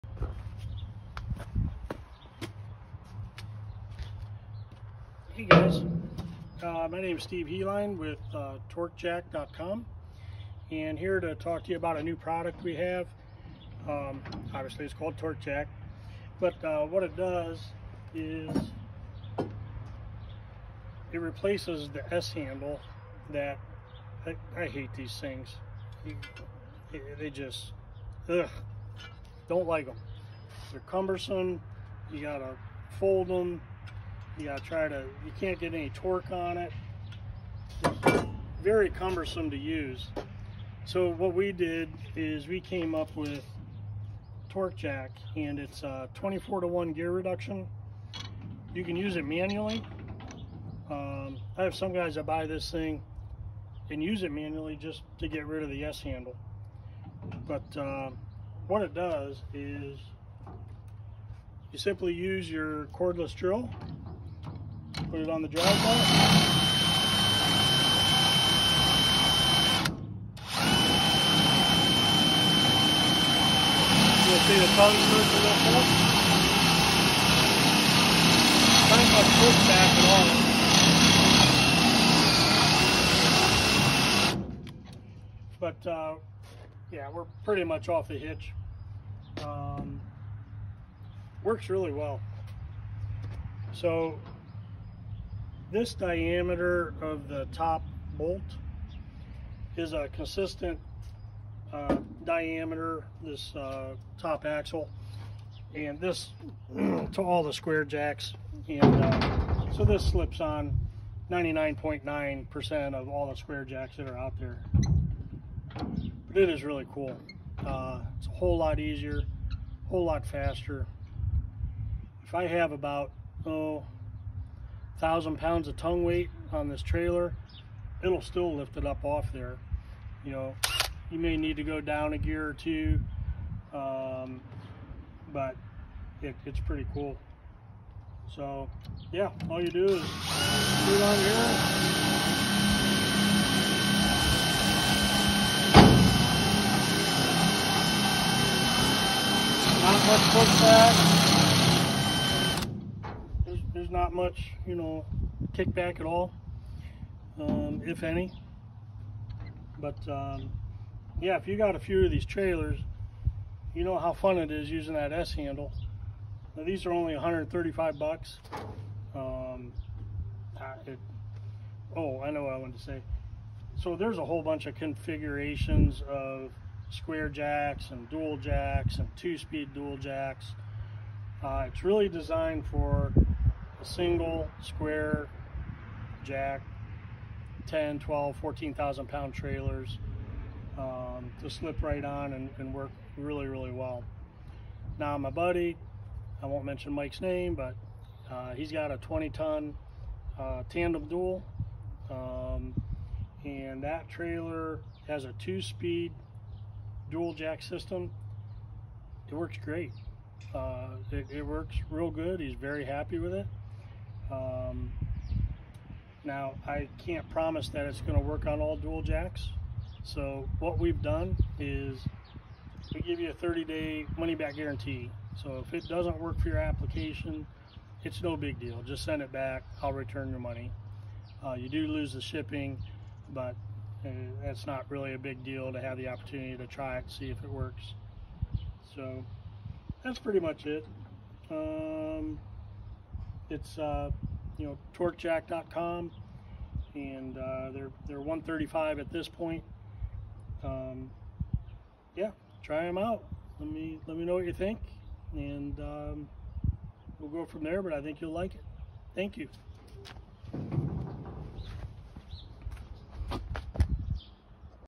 Torquejack.com
Torque jack assembly to replace the dreaded s handle found on typical trailers. Use your cordless drill to operate or run it by hand.